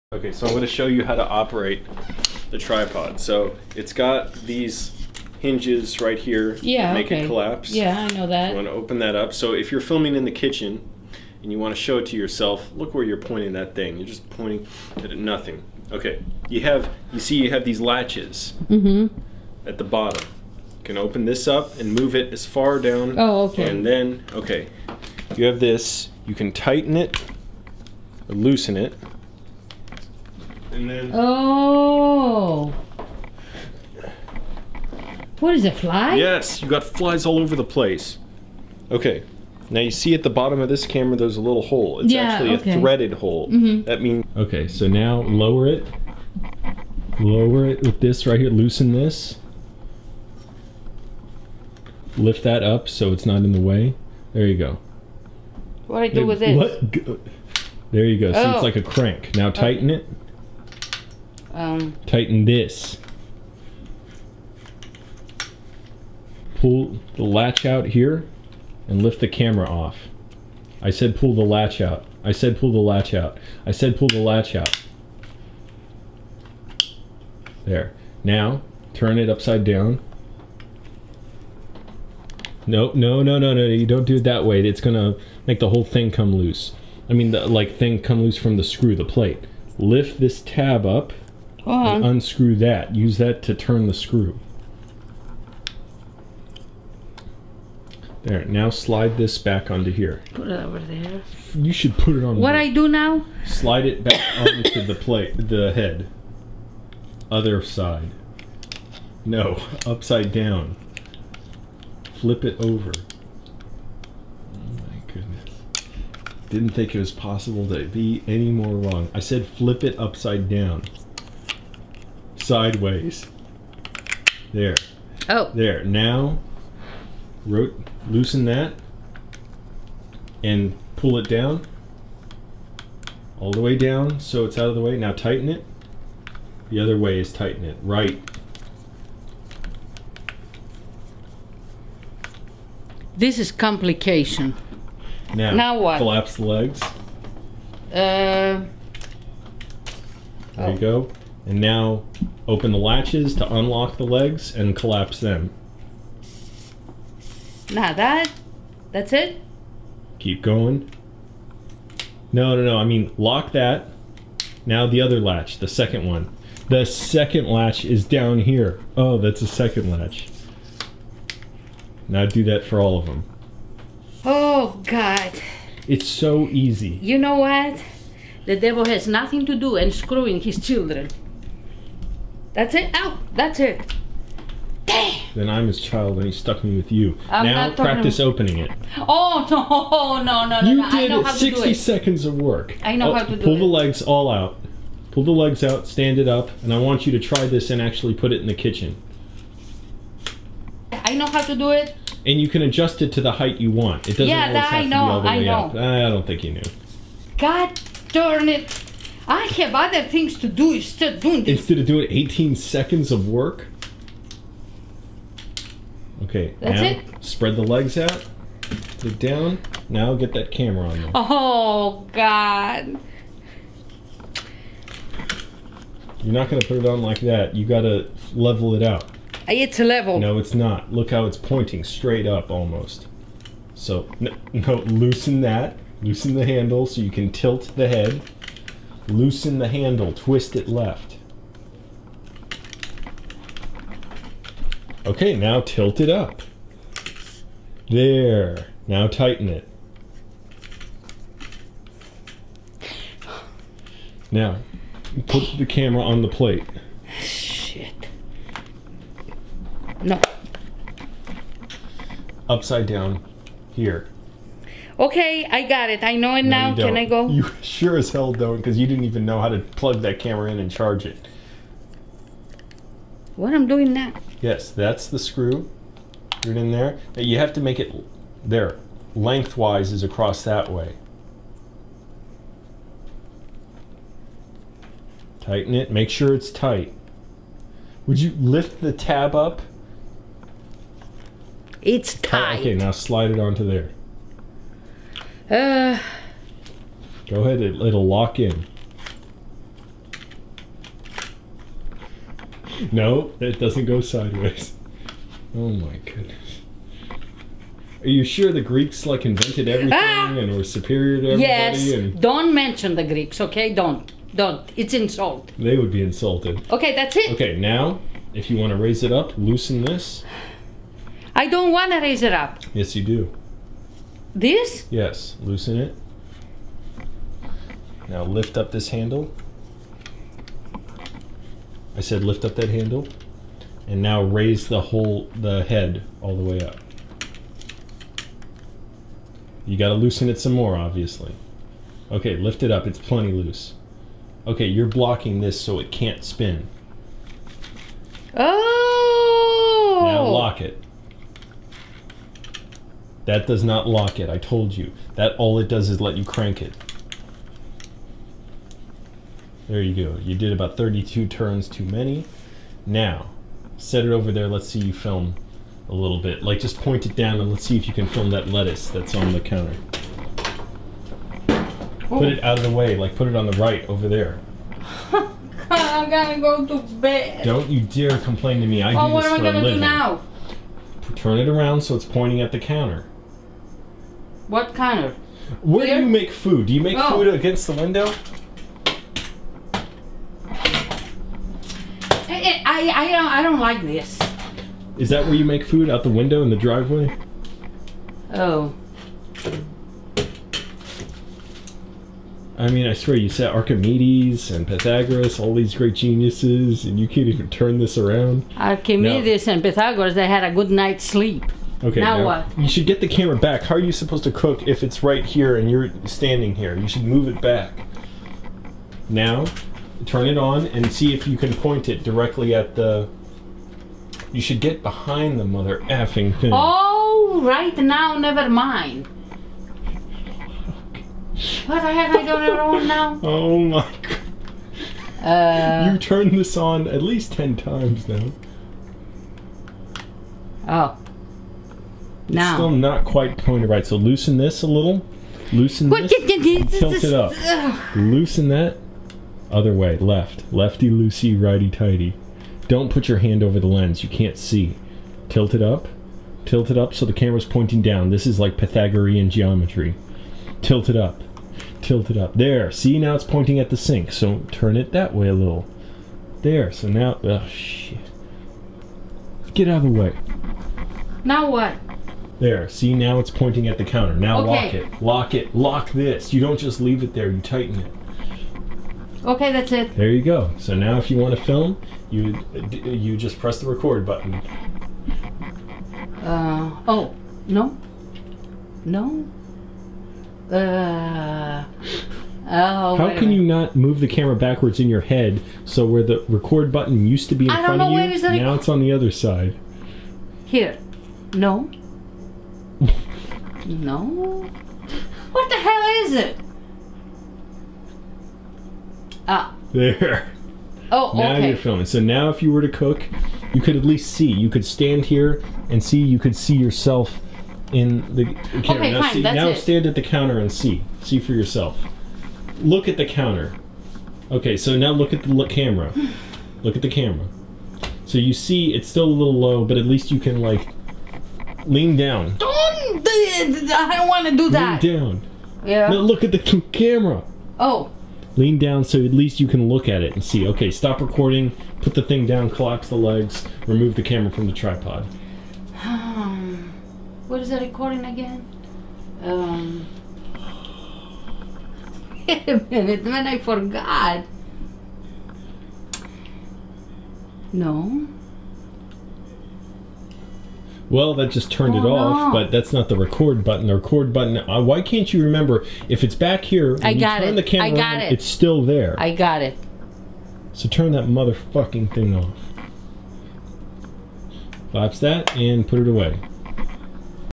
Angelo's Mom Learns to Use a Tripod (Barely)
My mom can't learn anything. I try to teach her how to operate a tripod so that she can better film herself and she practically acts like I'm terrorizing her with a prank and immediately forgets everything she learns.

What she really needs to do is pull the latch out. Pull the latch out. I said, pull the latch out.